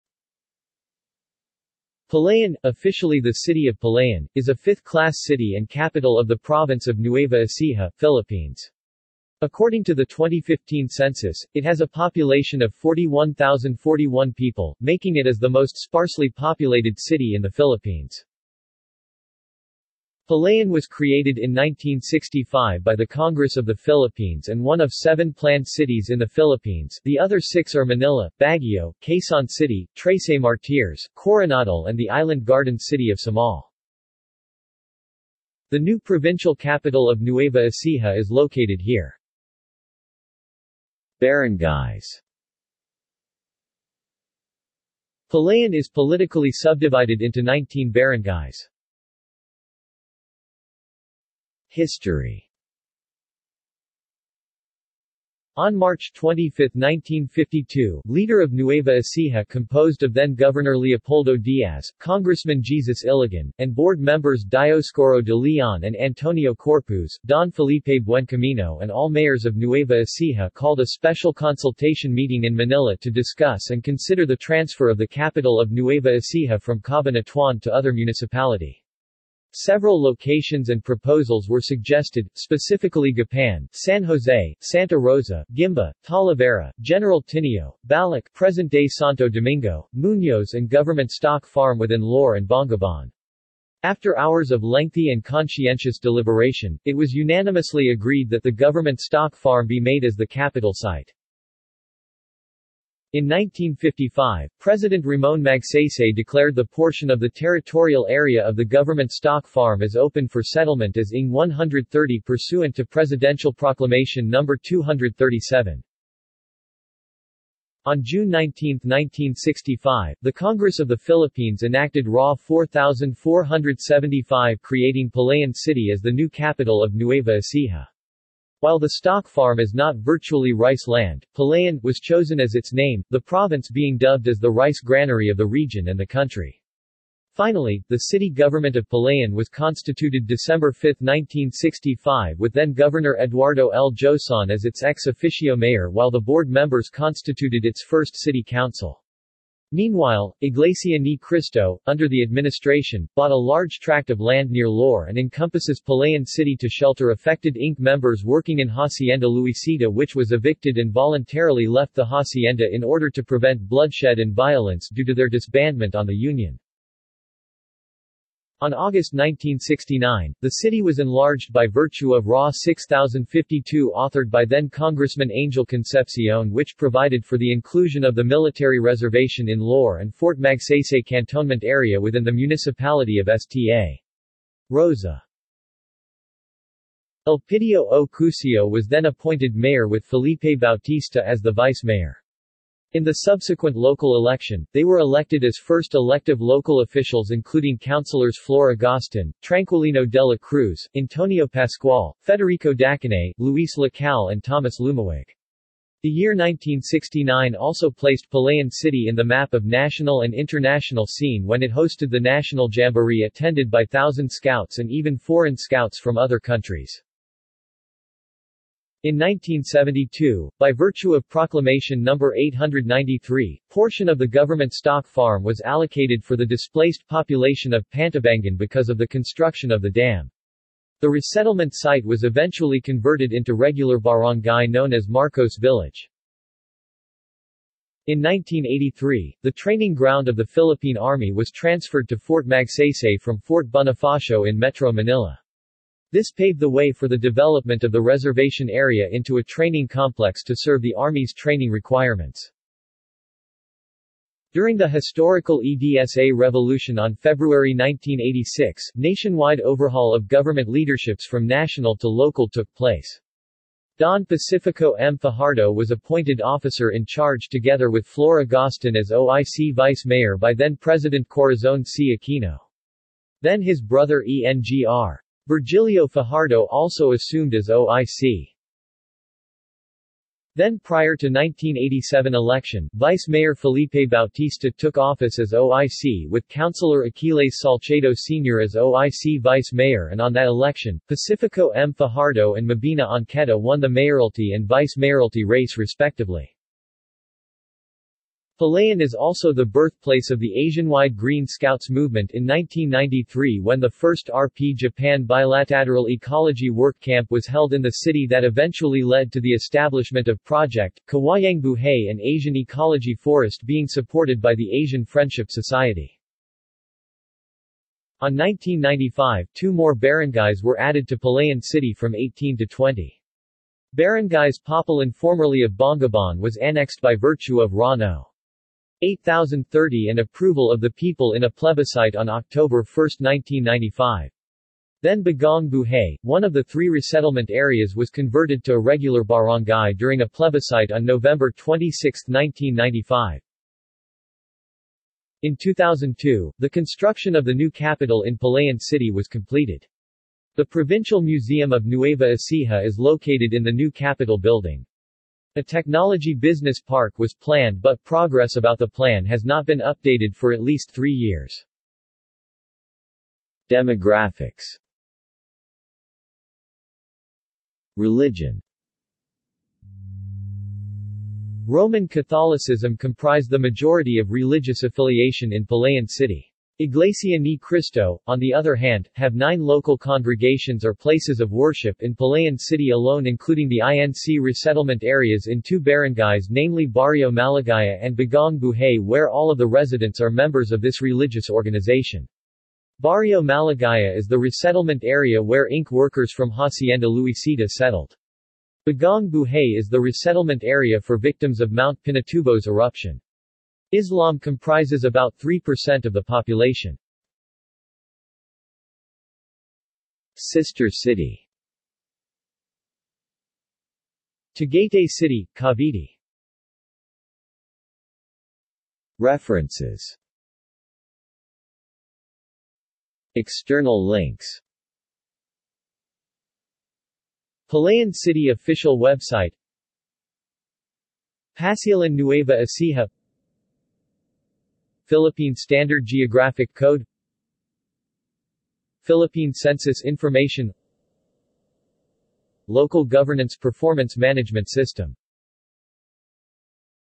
Palayan | Wikipedia audio article
This is an audio version of the Wikipedia Article:
Palayan

SUMMARY
Palayan, officially the City of Palayan,  is a 5th class city and capital of the province of Nueva Ecija, Philippines. According to the 2015 census, it has a population of 41,041 people,  making it as the most sparsely populated city in the Philippines.
Palayan was created in 1965 by the Congress of the Philippines and one of seven planned cities in the Philippines (the other six are Manila, Baguio, Quezon City, Trece Martires, Koronadal and the Island Garden City of Samal).
The new provincial capitol of Nueva Ecija is located here.